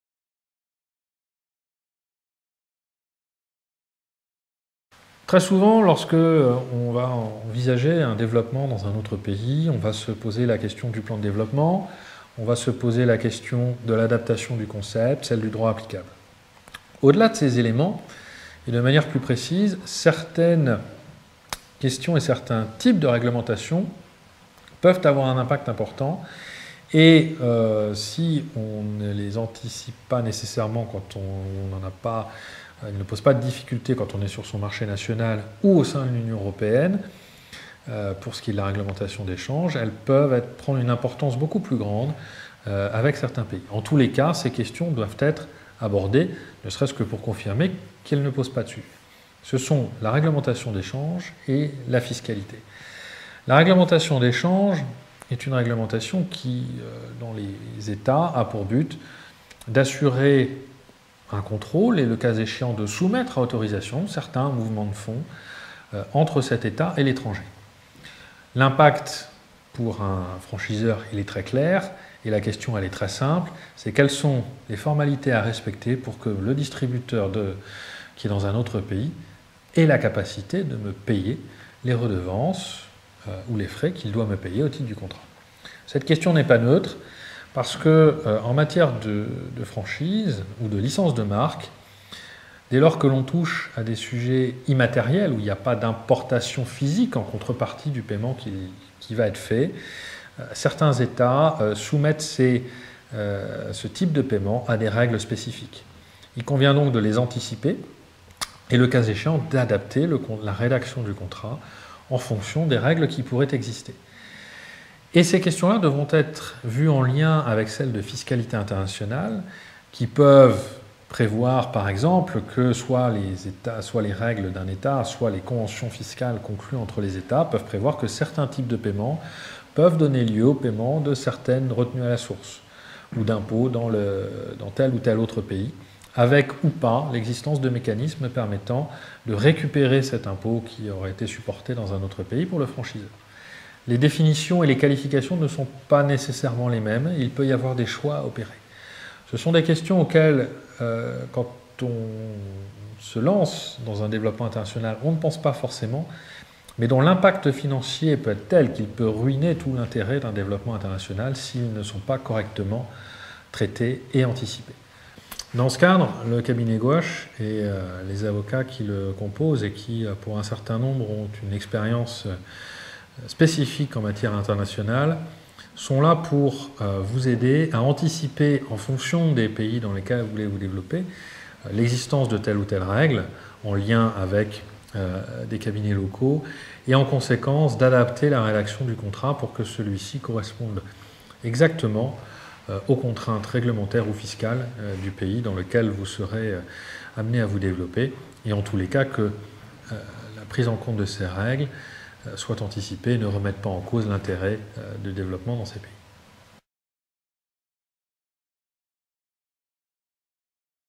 Se développer à l'étranger : règlementation des changes et fiscalité
Lorsqu'une tête de réseau de distribution (franchiseur, concédant d'une licence de marque, etc...) envisage un développement international, il est nécessaire de vérifier que le rendement net du contrat sera effectif :

- la réglementation des changes dans le pays visé ne doit pas empêcher le rapatriement des fonds en France ; si elle soumet à autorisation le versement effectif des sommes à la tête de réseau, il conviendra d'anticiper les conditions d'octroi de l'autorisation ;

- la fiscalité peut également impacter le revenu de la tête du réseau de distribution : il convient d'anticiper d'éventuelles retenues à la source selon les règles de fiscalité domestique de l'Etat où est situé le distributeur, et les modalités éventuelles d'élimination des doubles imposition.

Gouache avocats pratique régulièrement la rédaction de contrats internationaux, en coordination avec des confrères étrangers.